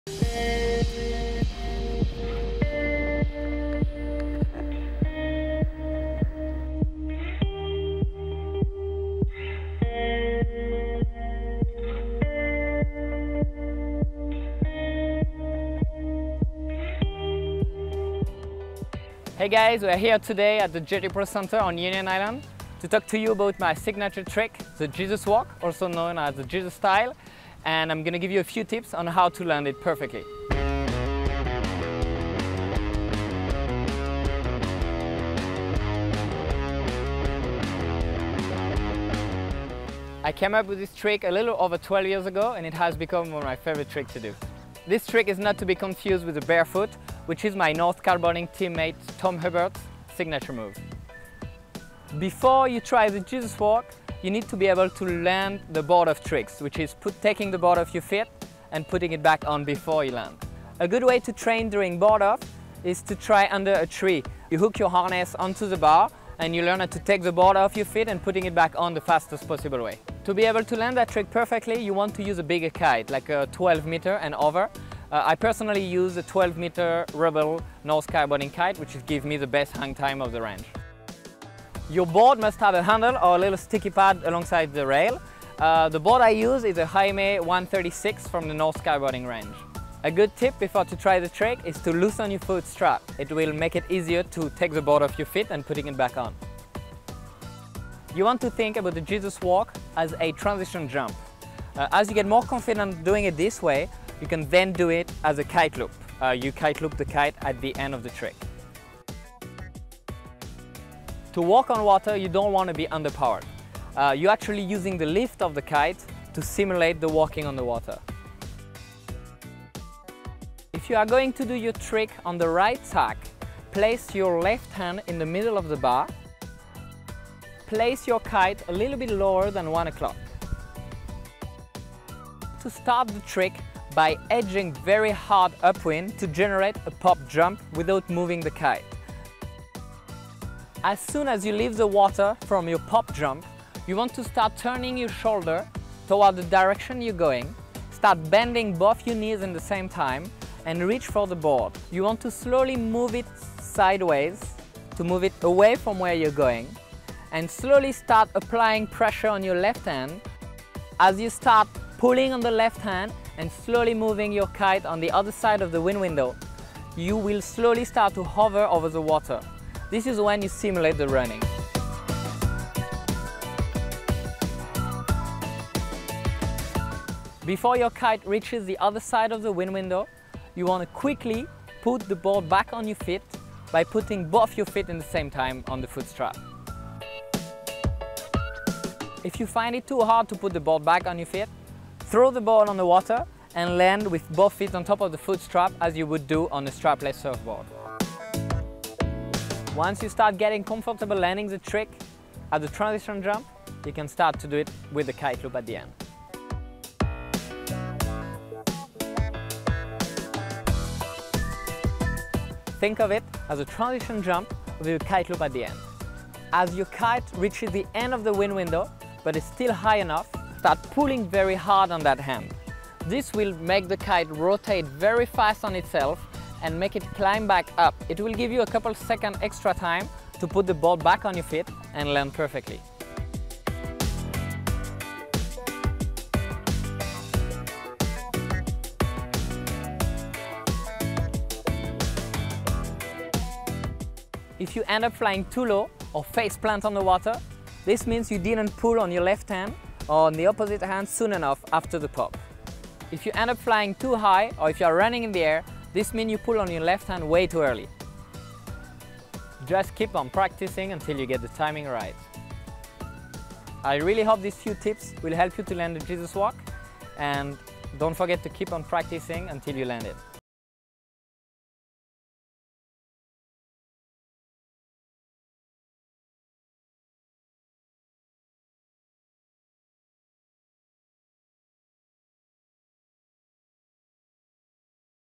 Kitesurfer Jeremie Tronet Breaks Down His 'Jesus Walk' Trick | Kite Masters | EDGEsport
Pro Kitesurfer Jeremie Tronet talks us through how he does his signature trick: the 'Jesus Walk'.